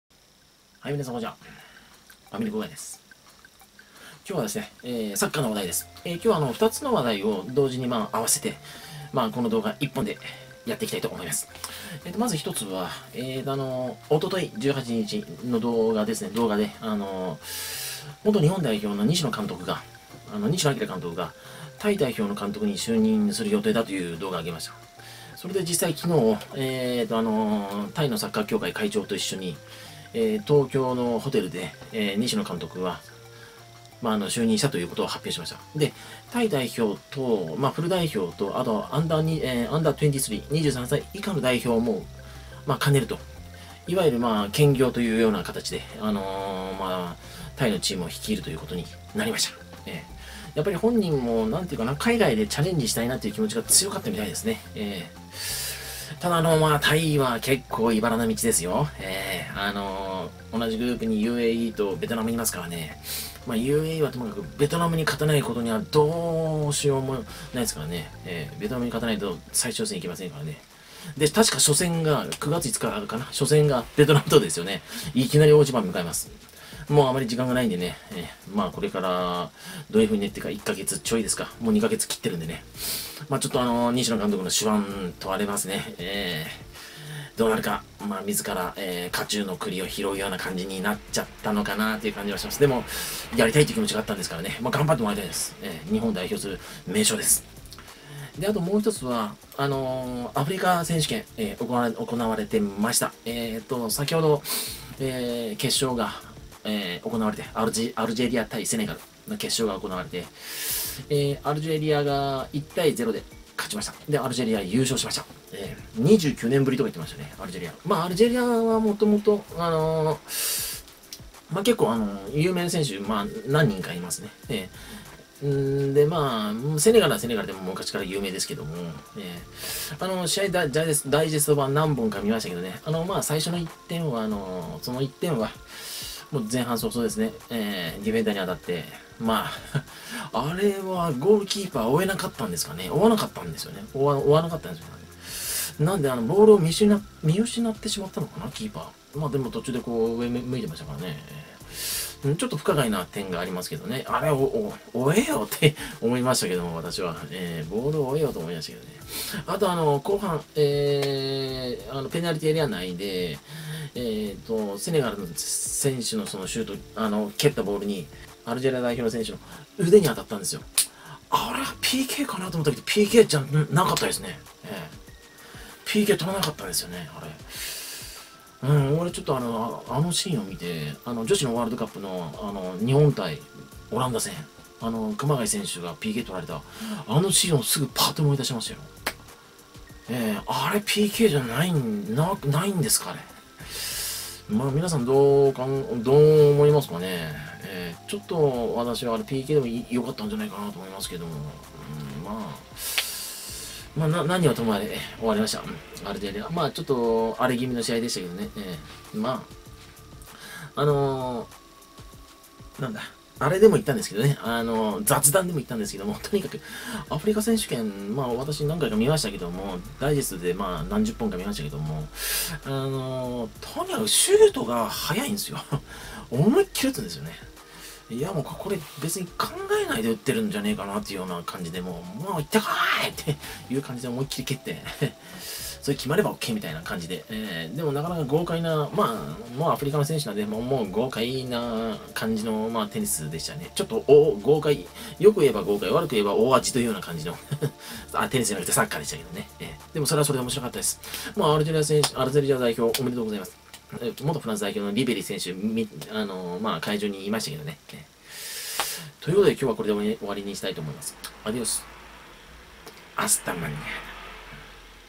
【サッカー】西野氏タイ代表監督に正式に決定、そしてアフリカ選手権決勝で、アルジェリアがセネガルに1-0で勝ち優勝を果たす！
19日にサッカー元日本代表監督の西野朗氏が、正式にタイ代表監督に就任しました。併せてU－23代表監督も兼任するとのことです。
そしてアフリカ選手権決勝で、アルジェリア代表がセネガル代表に1-0で勝ち優勝を決めました。
今回の動画は、この二つの話題を合わせて1つの動画として編集しました。